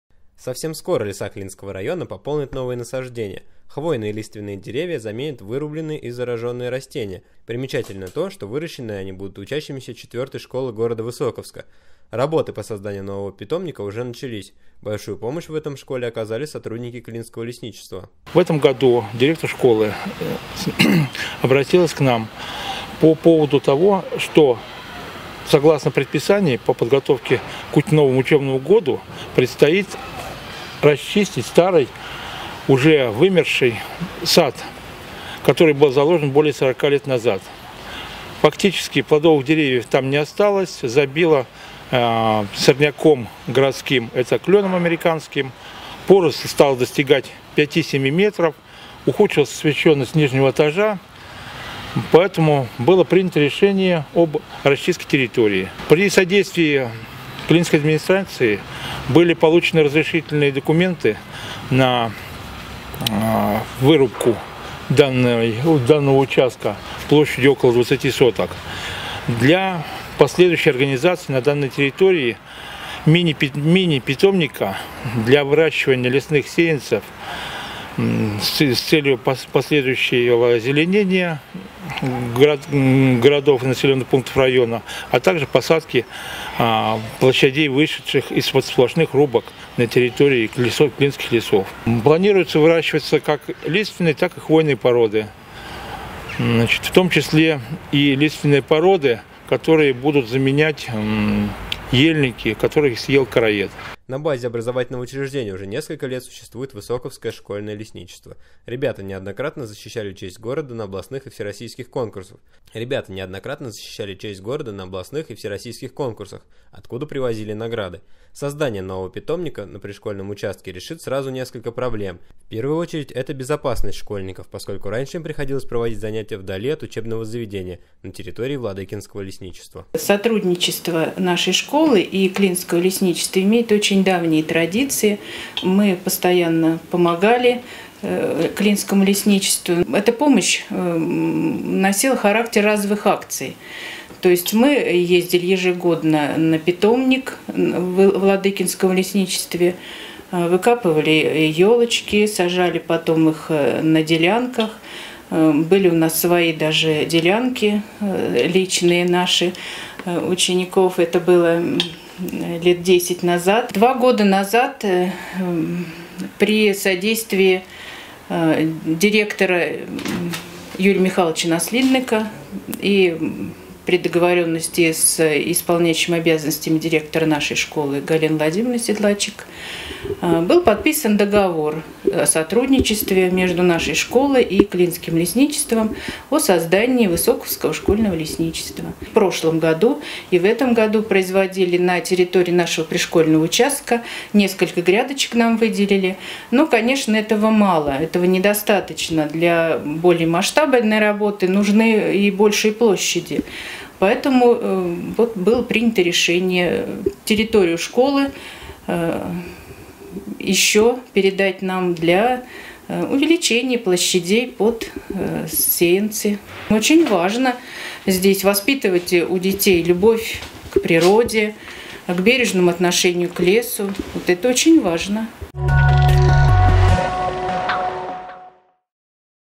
Новый питомник в Высоковской школе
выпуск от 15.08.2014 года

Подготовка к учебному году в Клинских школах продолжается полным ходом. Где-то приводят в надлежащий вид внешний облик зданий, где-то внутреннее убранство, а в четвертой школе города Высоковск готовятся создать новый питомник.